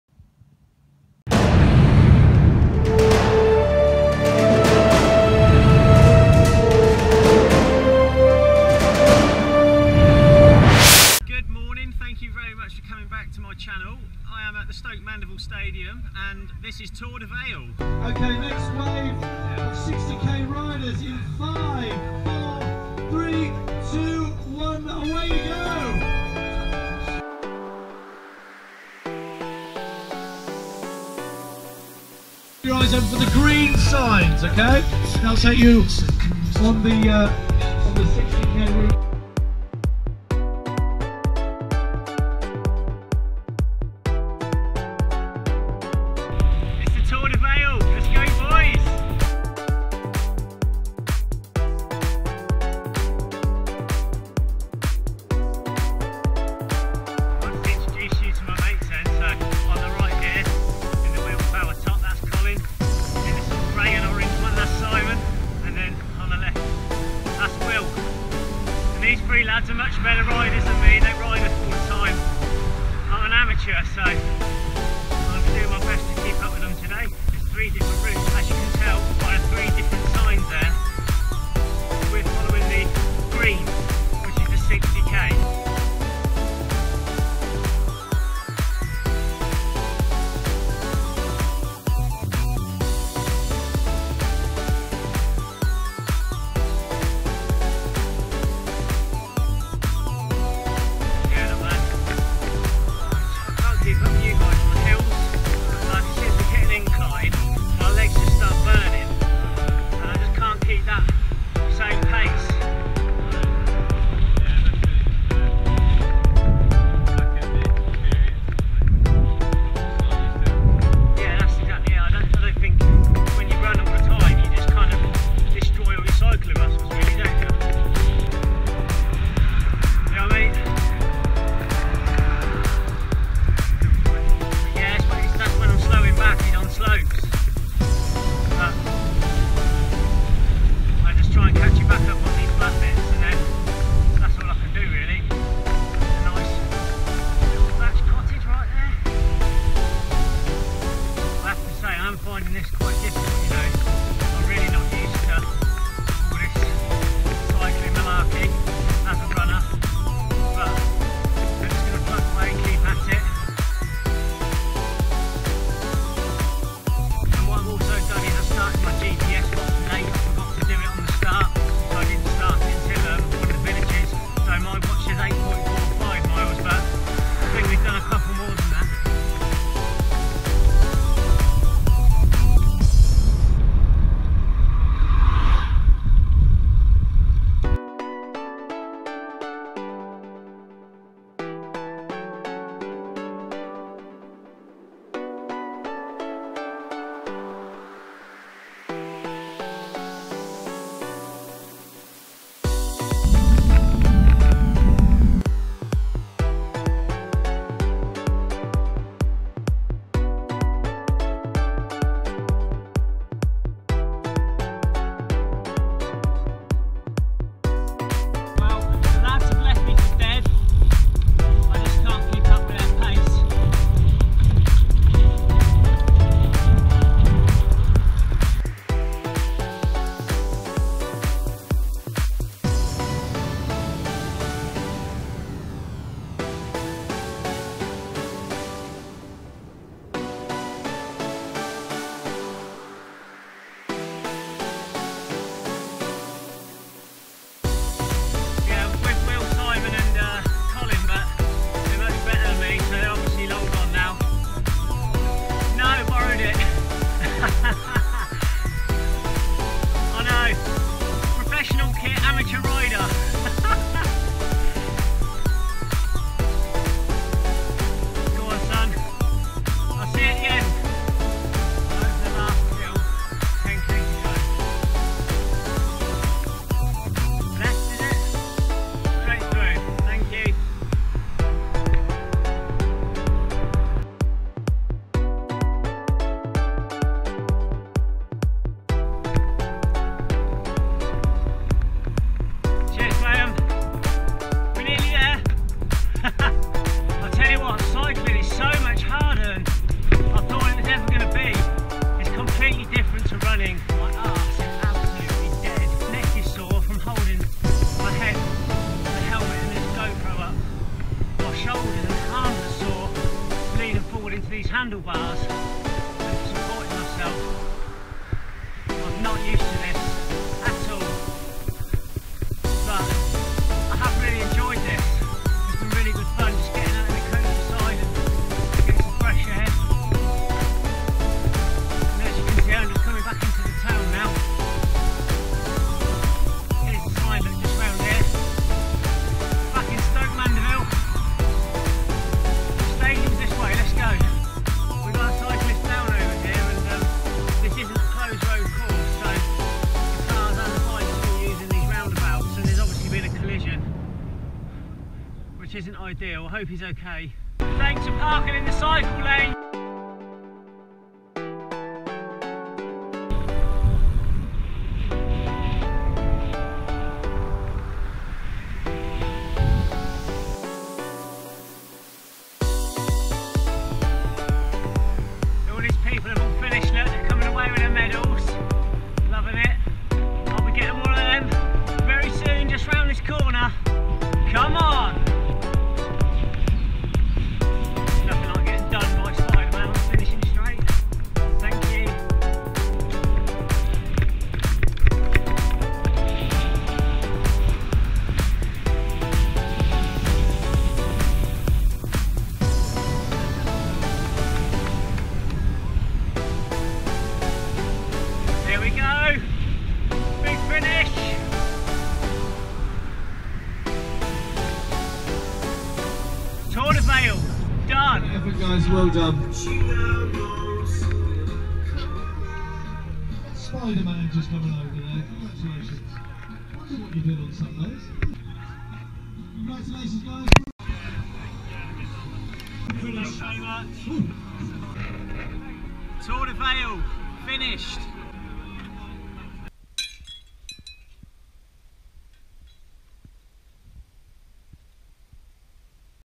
Tour De Vale 2016
I take on the Mix 96 Tour De Vale bike ride which raises money for Wheel Power. Setting off from the home of the Paralympics Stoke Mandeville Stadium